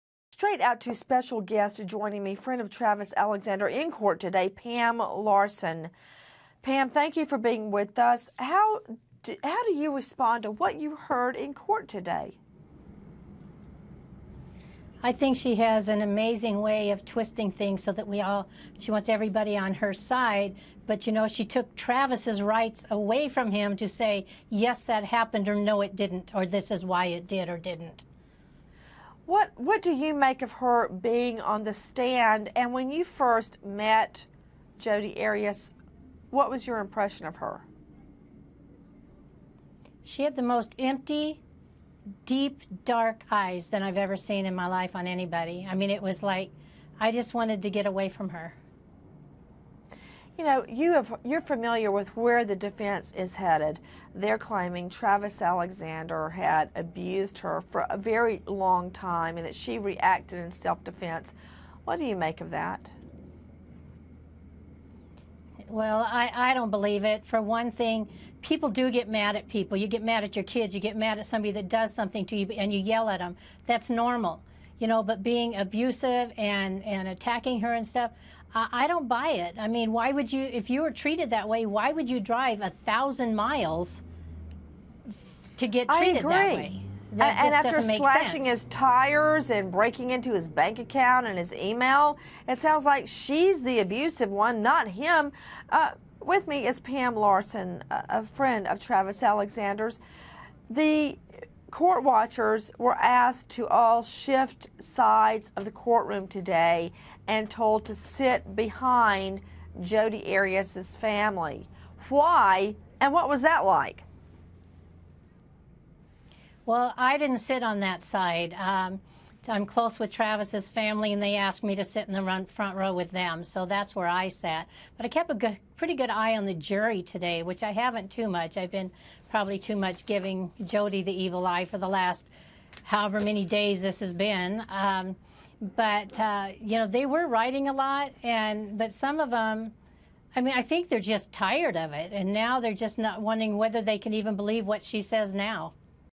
Travis' friend: Arias has 'empty, deep, dark eyes'
Nancy Grace speaks to a friend of Travis Alexander about the testimony of Jodi Arias, who today began describing the night of his murder.